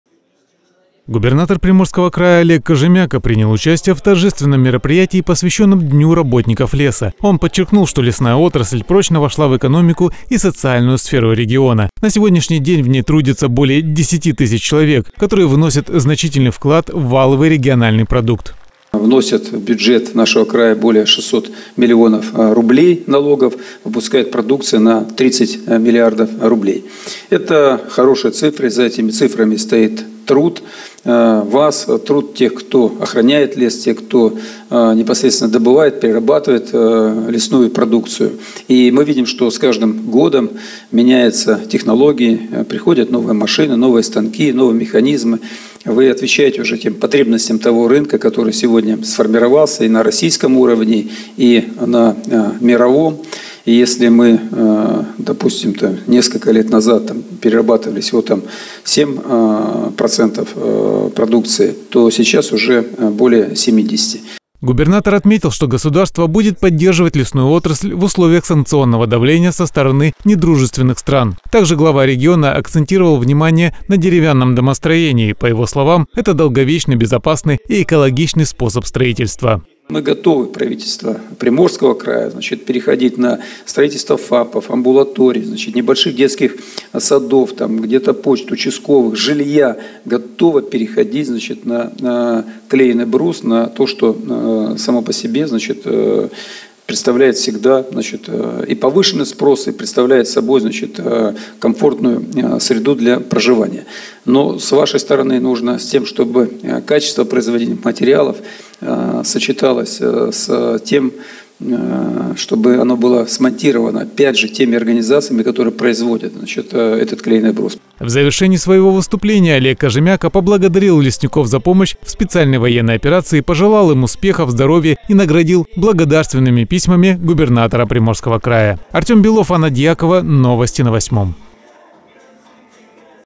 «День работников леса»
Губернатор Приморского края Олег Кожемяко принял участие в торжественном мероприятии, посвященном Дню работников леса. Он подчеркнул, что лесная отрасль прочно вошла в экономику и социальную сферу региона. На сегодняшний день в ней трудится более 10 тысяч человек, которые вносят значительный вклад в валовой региональный продукт.